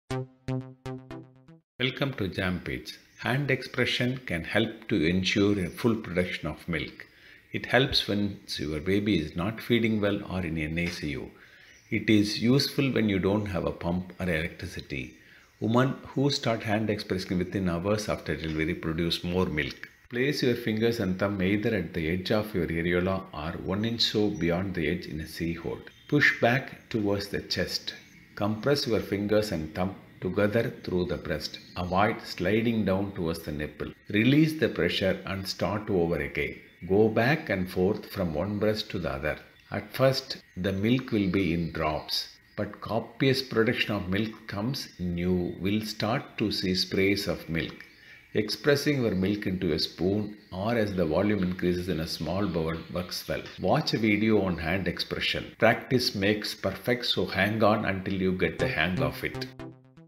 Hand expression
Expressing breast milk is an important skill essential for the mother whose babies are ill , small or preterm.Many people including  health care personal aur not trained enough in the art of expressing breast milk
this jam video  will help you to learn the correct technique